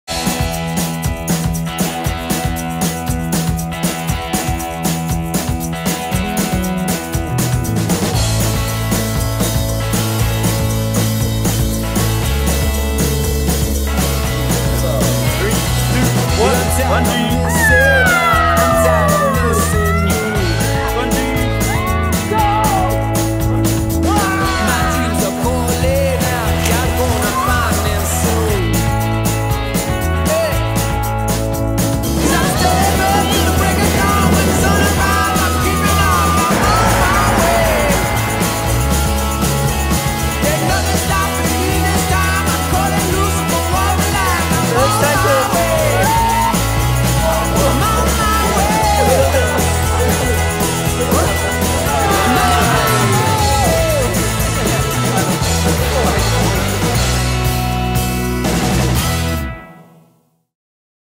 Queenstown - Bungeejumping und Bars
Erlebe einen fantastischen Tag in Queenstown. Hier kannst du Bungeespringen, Schlitten fahren, Fallschirm springen, die heilende Wirkung einer heißen Quelle genießen und später in den aufregenden Bars die Nacht zum Tag machen.